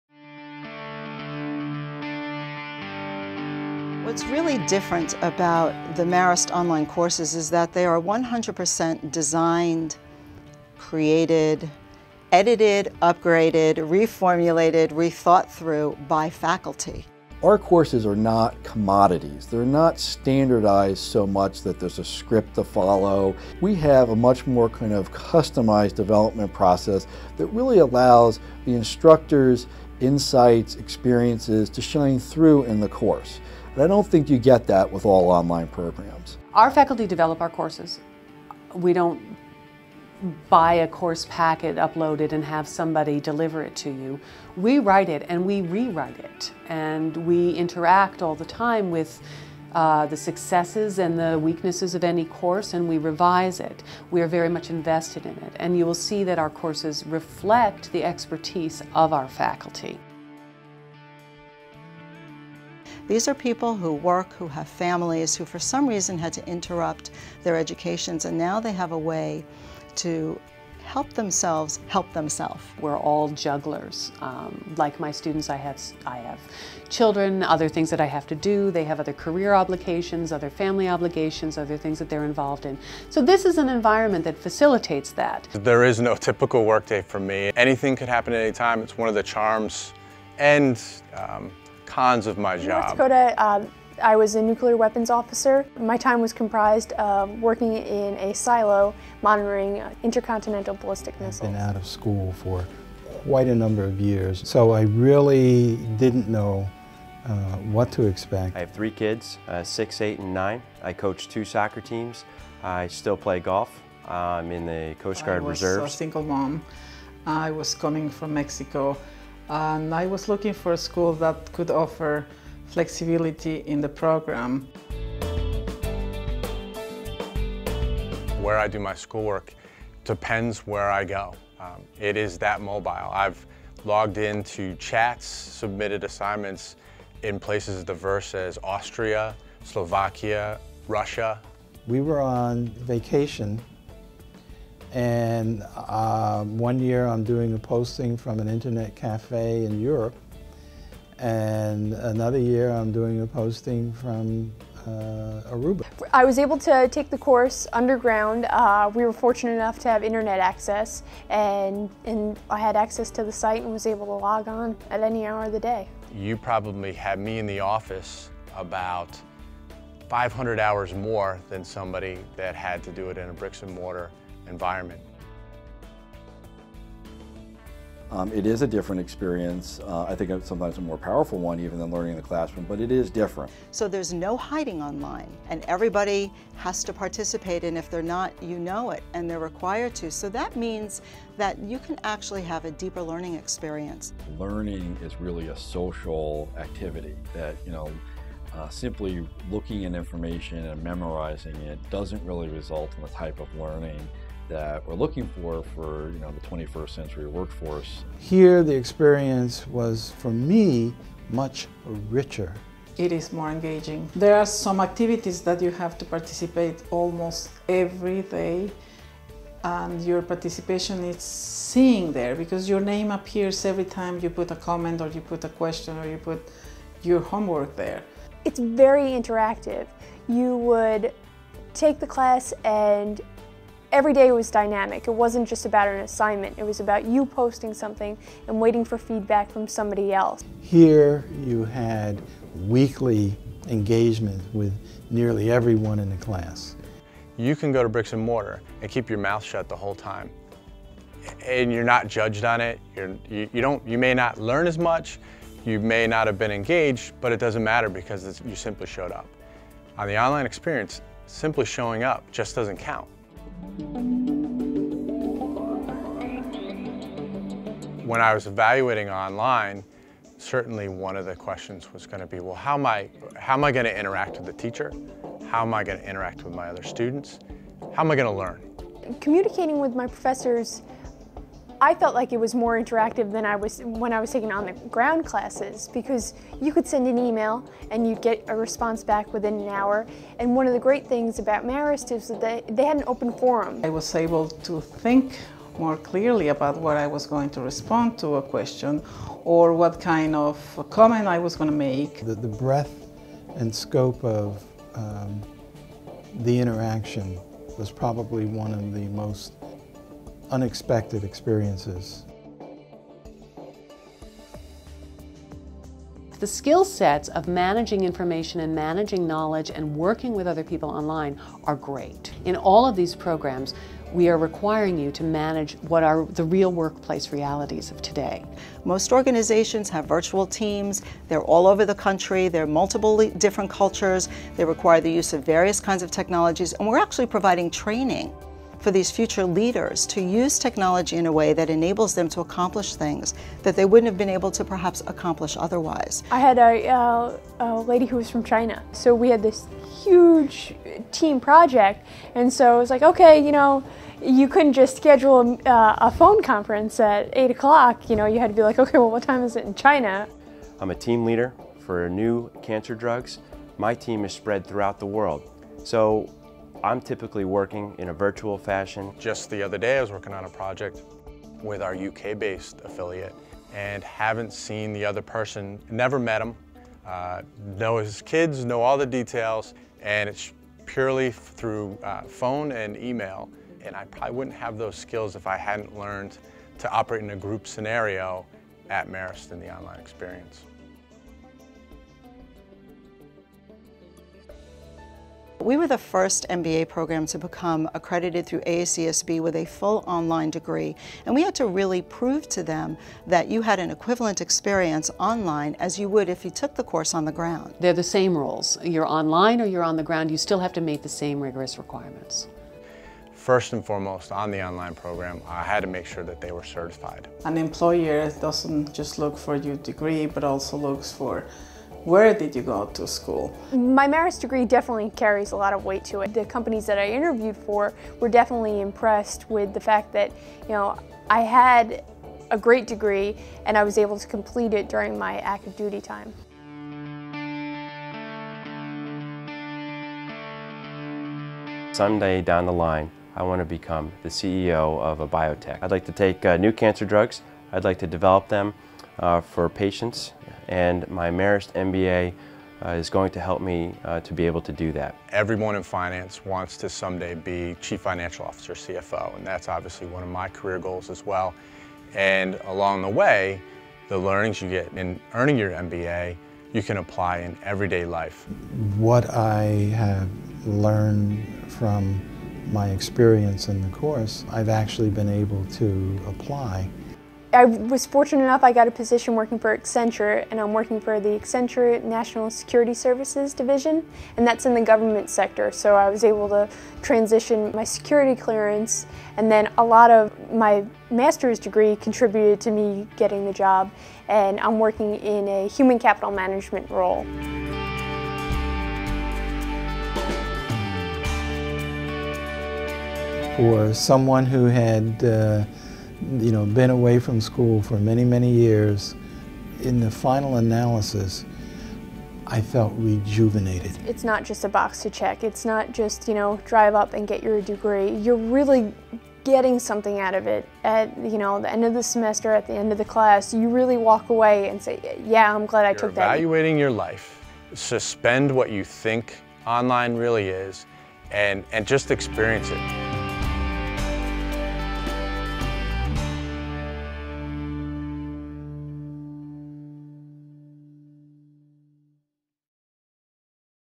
Marist College Online Graduate Programs
Faculty gives a comprehensive overview of the online Graduate Programs at Marist College.